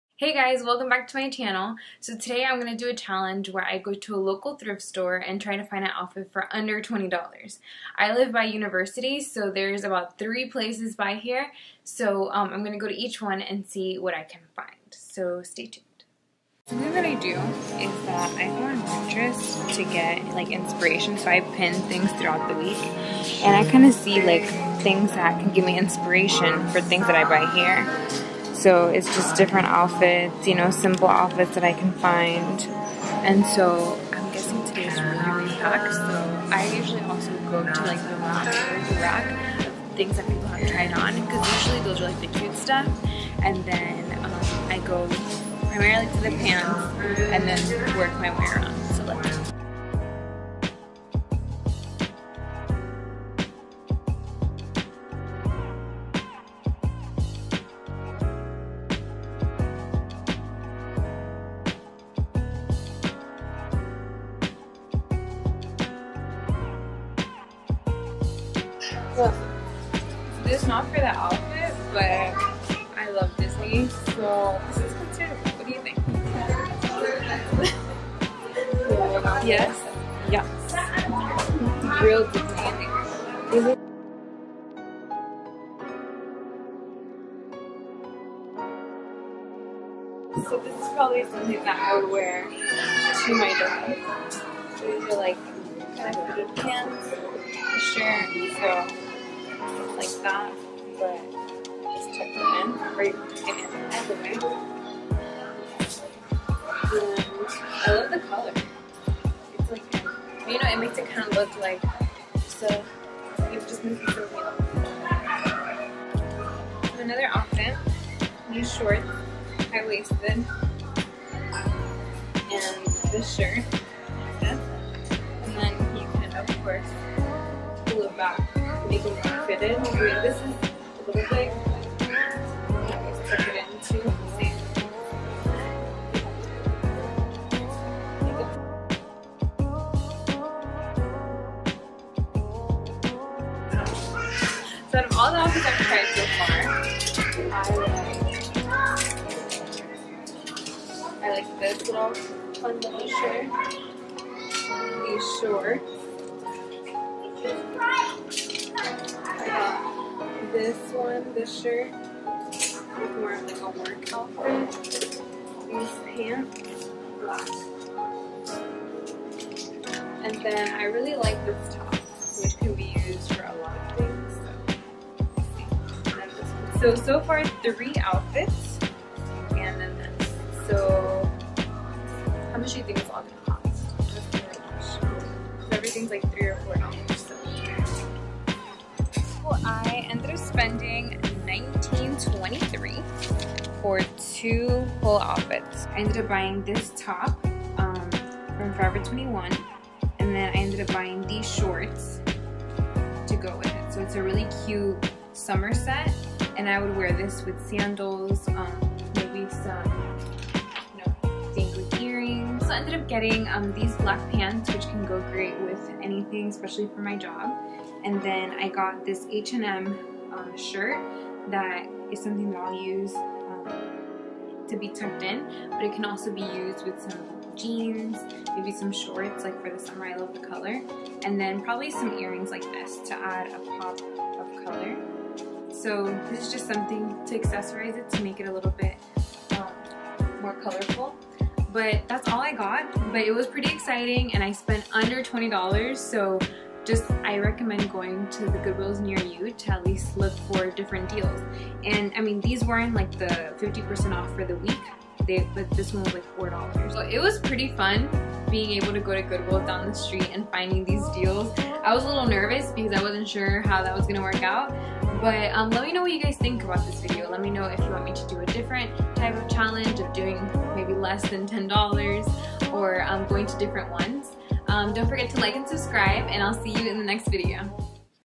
Thrifting Haul Outfit Under $10
Hey guys, I did an impromptu thrifting at Goodwill and found some outfits under $10. Let me know what you think.

Follow me @elizabethsabby
xoxo